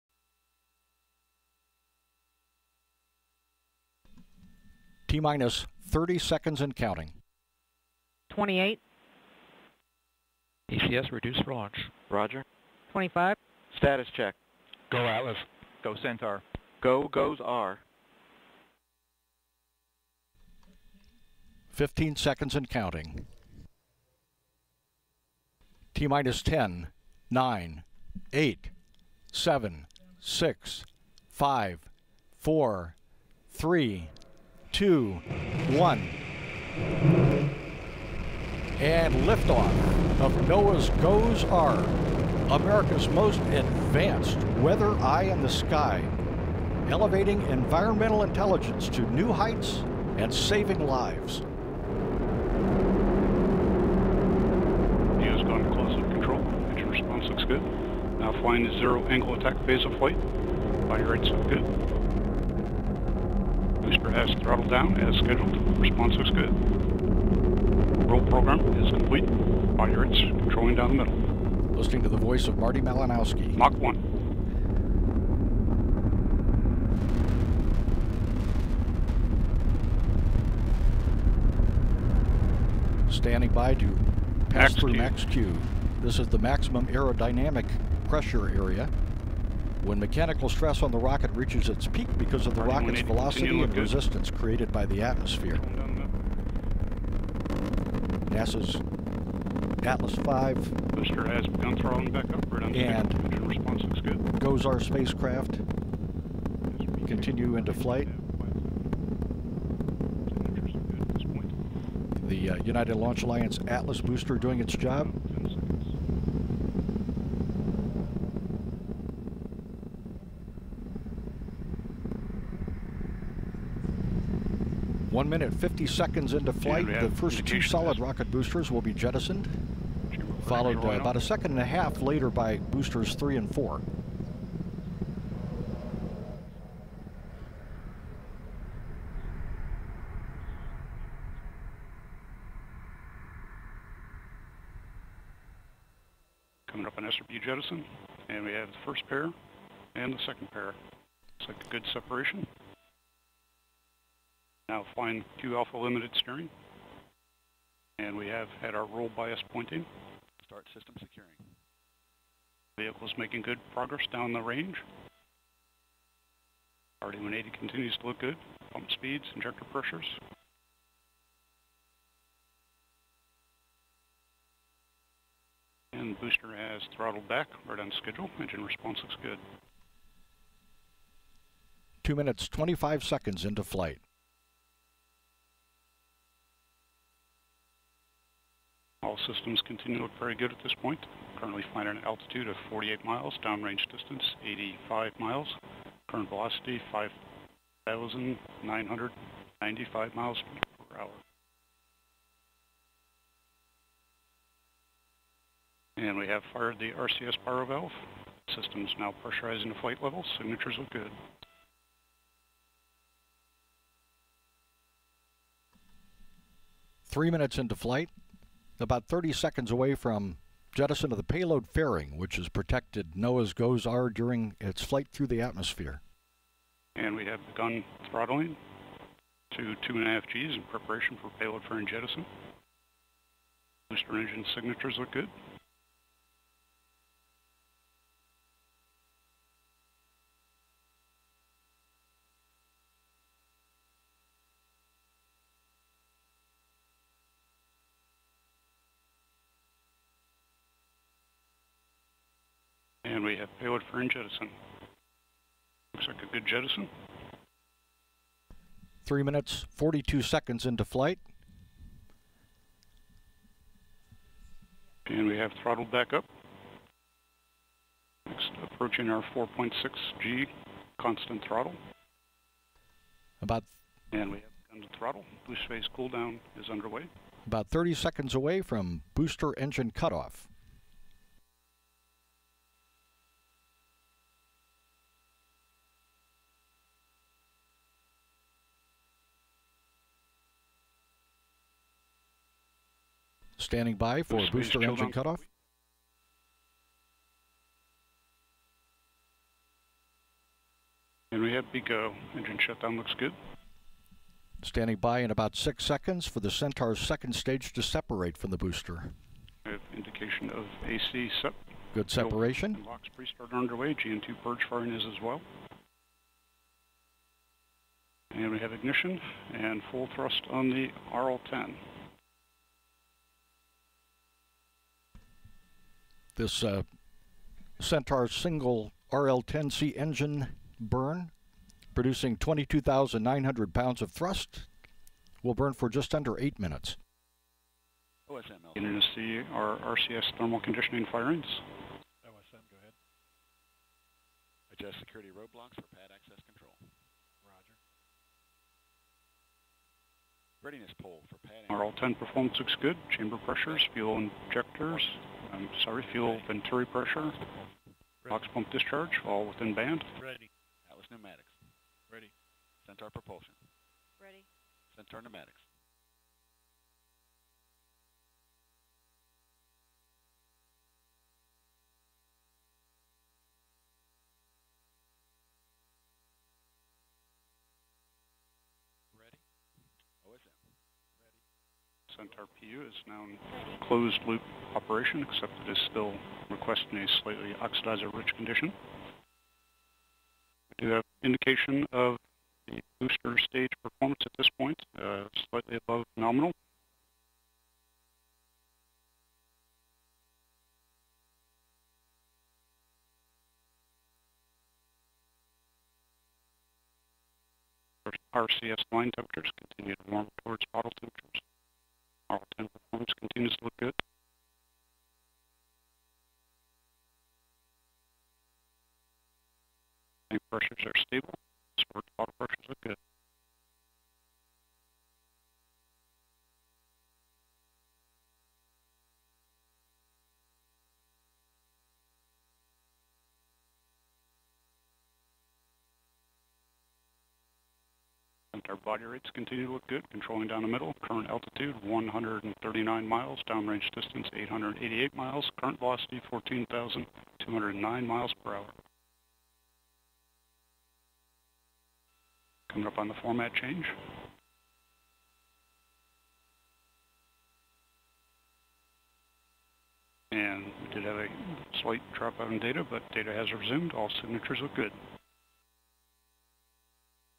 GOES-R Weather Satellite Launched Atop Atlas V Rocket | Video
The NOAA satellite was launched by the United Launch Alliance rocket on Nov. 19, 2016 from Cape Canaveral Air Force Station in Florida. The satellite will be a 'game changer' for weather forecasting. -- Full Story

Credit: NASA